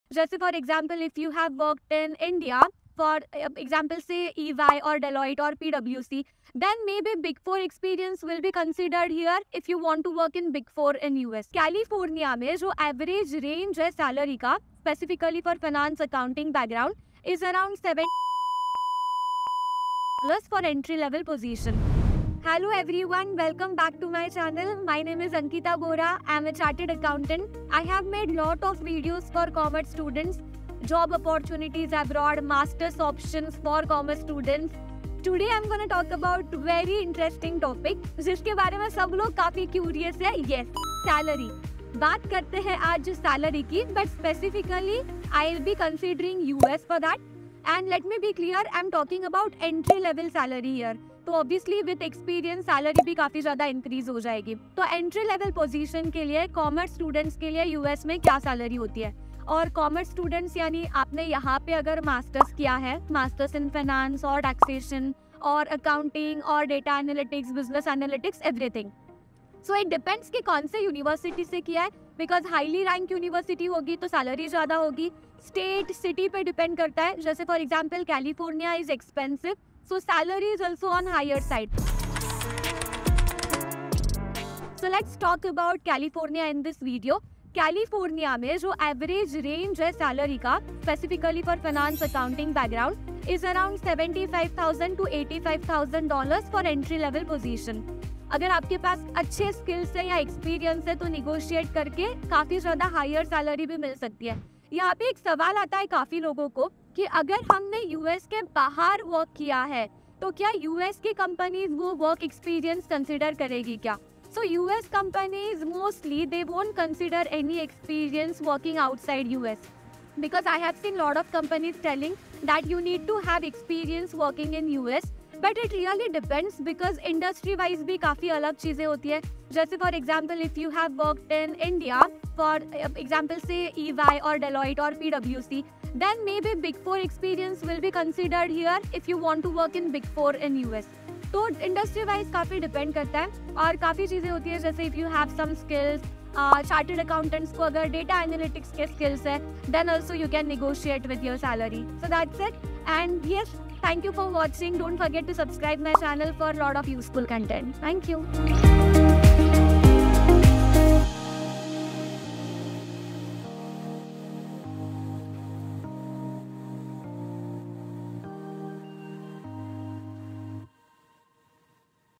Salary range for commerce students in USA| CA Ankita Bora
Interested in a Commerce career in the US? Learn what you’re worth as a recent graduate or seasoned professional in this informative video from CA Ankita Bora. As a trusted source of financial education, Ankita Bora breaks down the average salary range for commerce students in the US, and explores what factors contribute to earning potential. Whether you’re planning to specialize in accounting, finance, or international business, knowing what you can earn is essential for making smart financial decisions. Check out this video from CA Ankita Bora and get inspired to take your Commerce career to the next level!